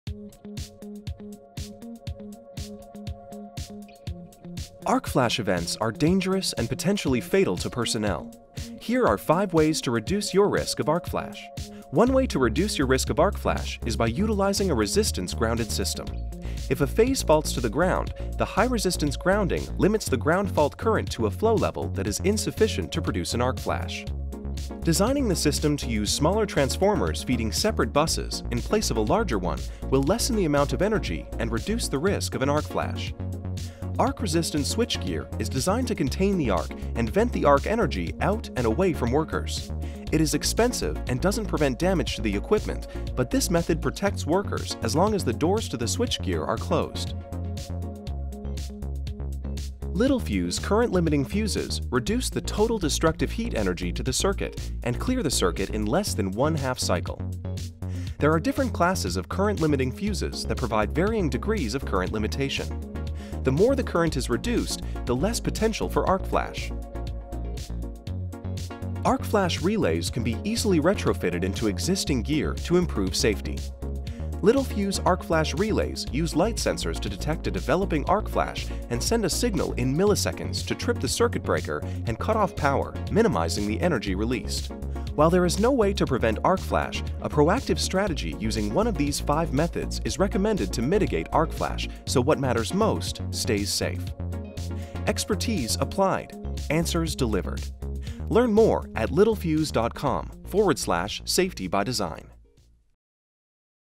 5 Ways to Reduce Your Risk of Having an Arc Flash
Arc-flash events are dangerous and potentially fatal to personnel. Here are five ways to reduce your risk of arc flash.